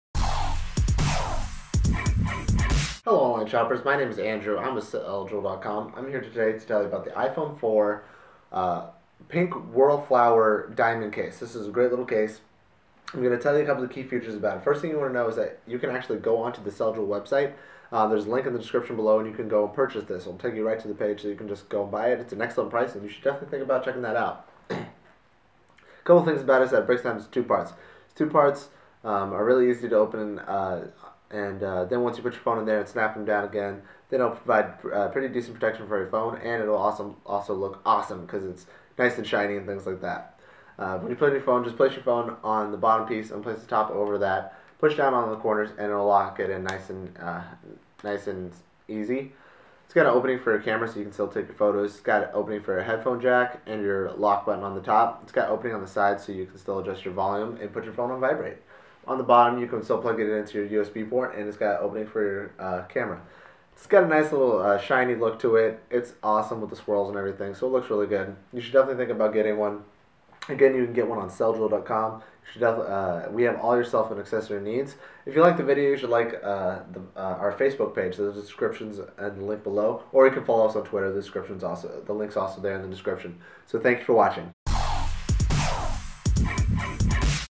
CellJewel.com - iPhone 4/4S Whirl Flower Pink & White Diamante Cover
The stylish design is complemented with rhinestones to add an air of glamor
Rhinestones embedded around giving a more luxurious look
Maximizes protection to your phone and allows direct access to all phone features